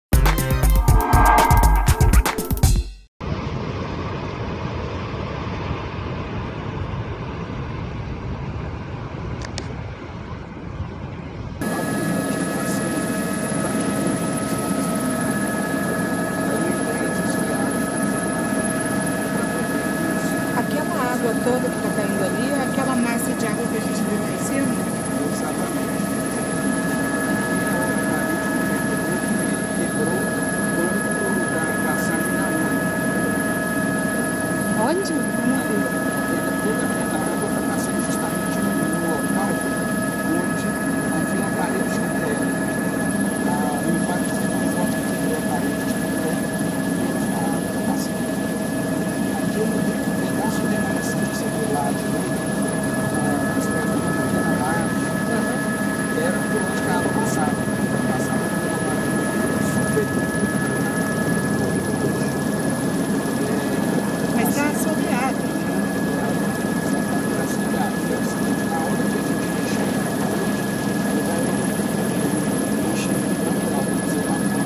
Barragem da Samarco em Mariana registra novo vazamento de resíduos
A Mineradora Samarco informou, por meio de comunicado, o registro de novo vazamento na Barragem de Fundão no último dia 27.